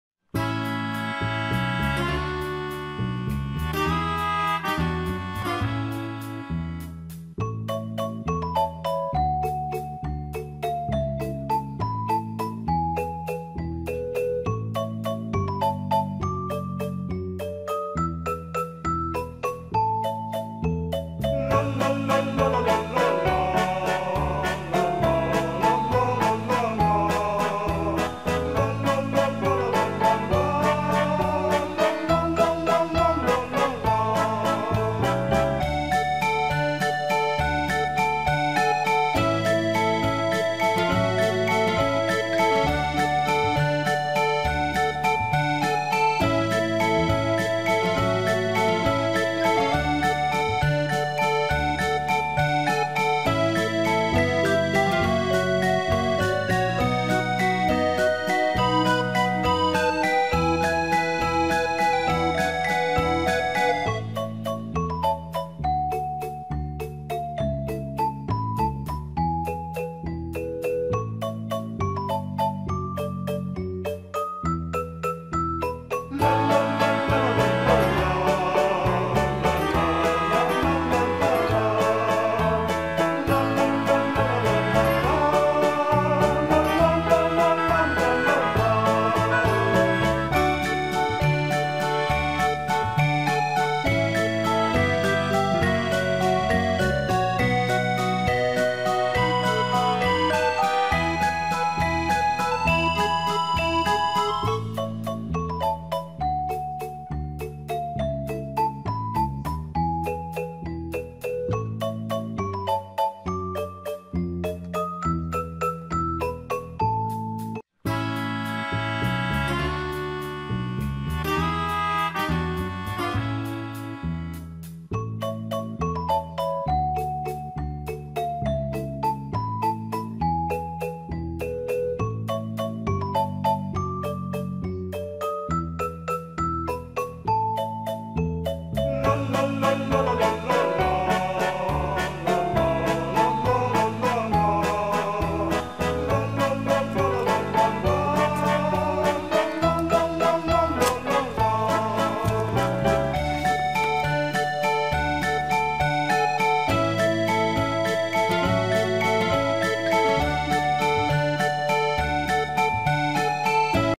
Unboxing TOYS Review/Demos - Part 1 Combi bathtub toy babies infant bathing companion education
Grandma toystore brings to you the unboxing demo/review of Part 1 Combi Floating bath tub toy babies infant bathing companion educational .  Your baby infant will love this toy as they watch how things float on water.